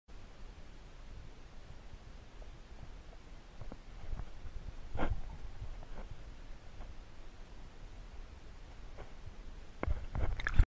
Crayfish Swimming in slow motion
shot on gopro in Catskill Creek, NY